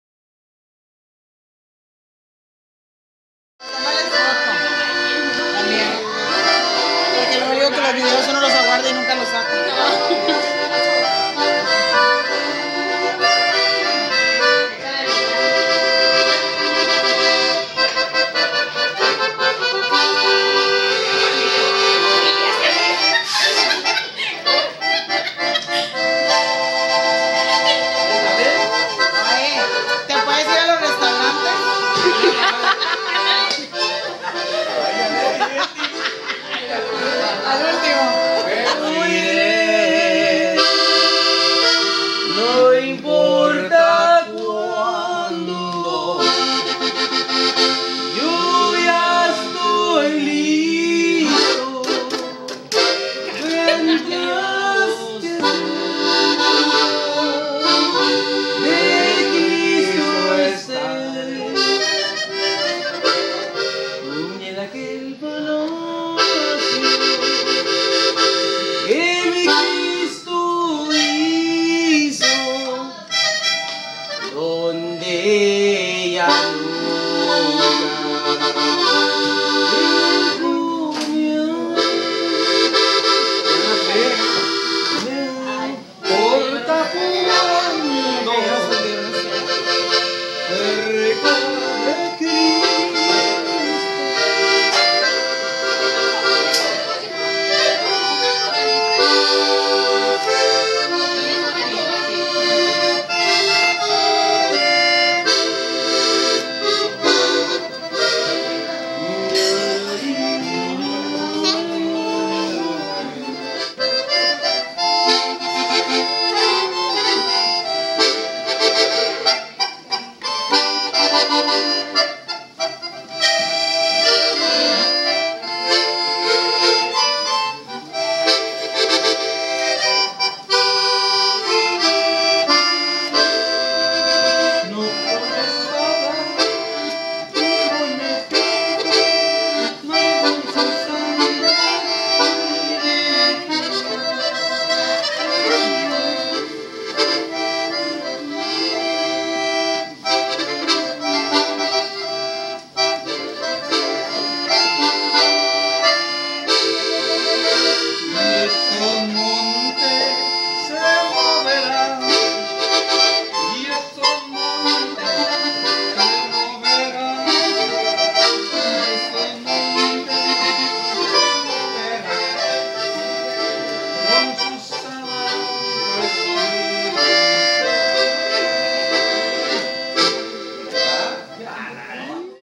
dad
My dad and my uncle Carlos playing on their accordions.